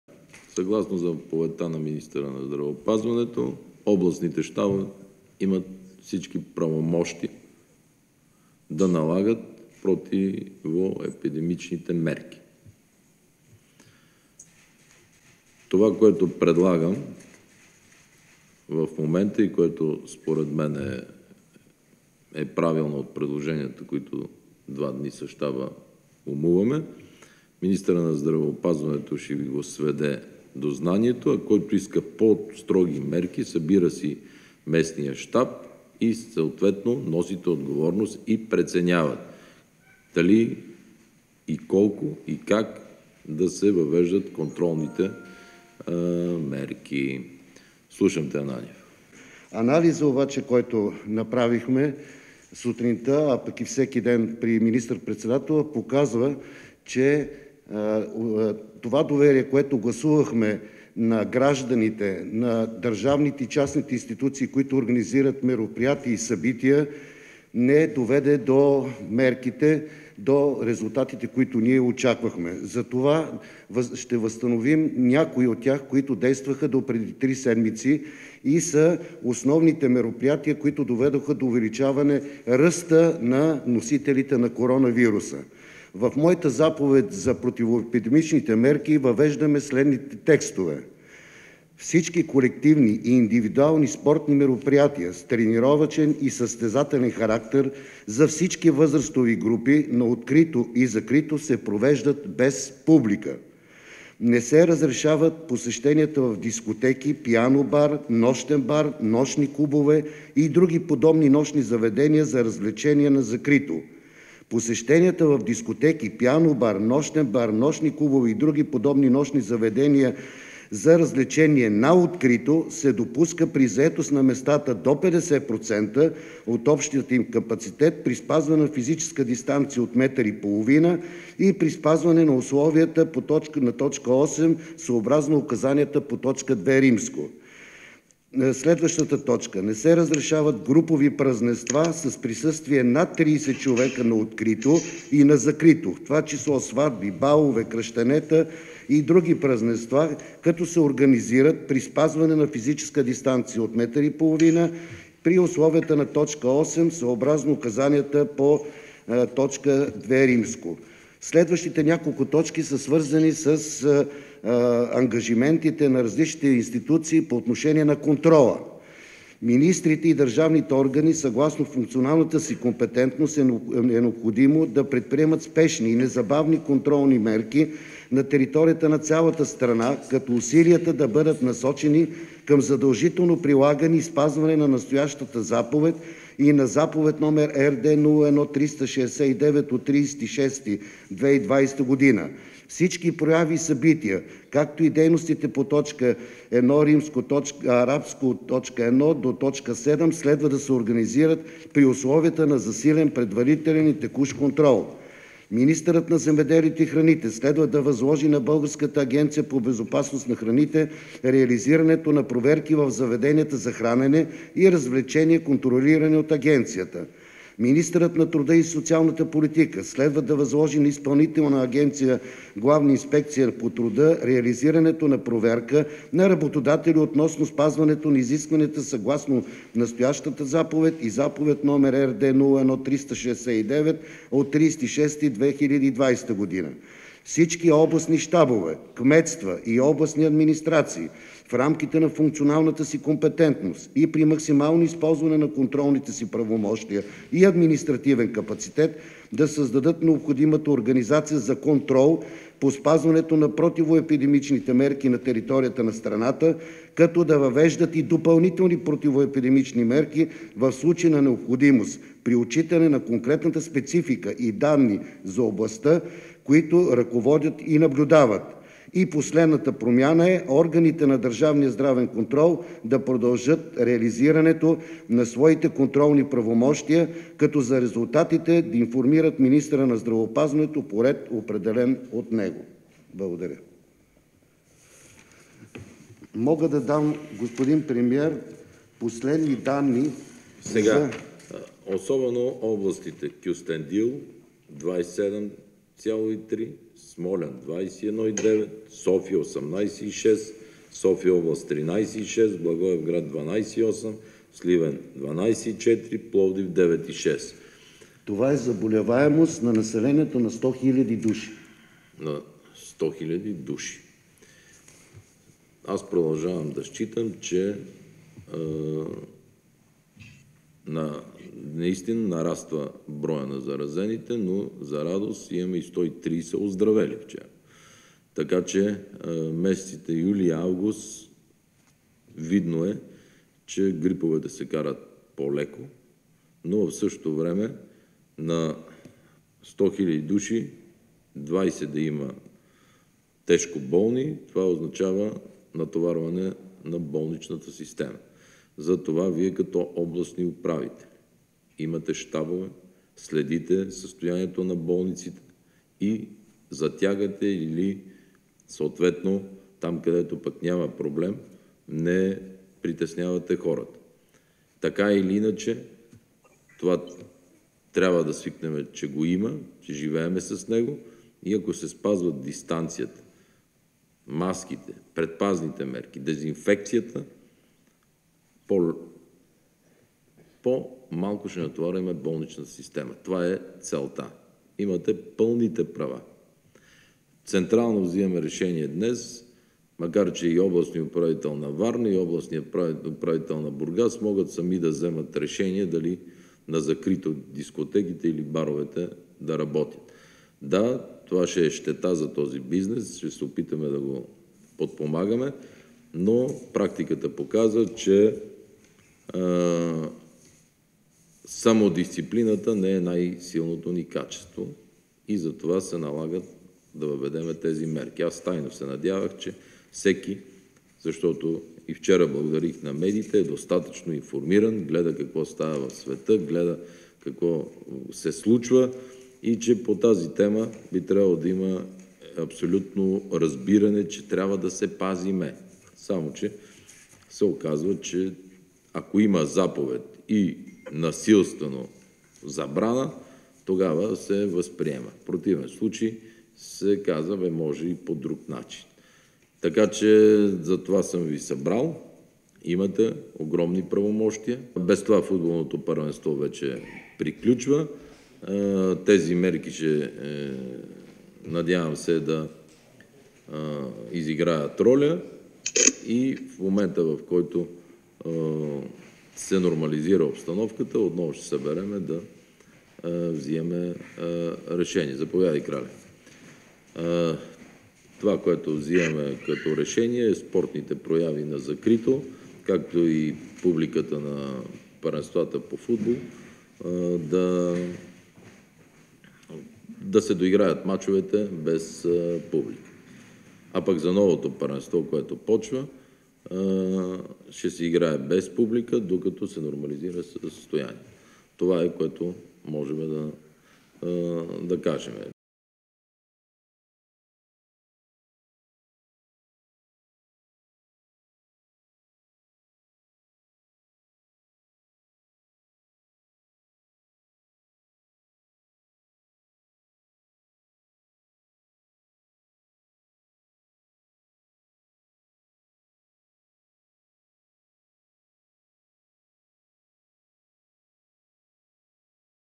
Бойко Борисов - 9 юли 2020 - COVID-19 - противоепидемични мерки
Всички спортни събития на открито и закрито ще се провеждат без публика, обяви министърът на здравеопазването Кирил Ананиев, а заповедта влиза в сила от петък и ще важи до второ нареждане.

Решението е съгласувано с министър-председателя Бойко Борисов.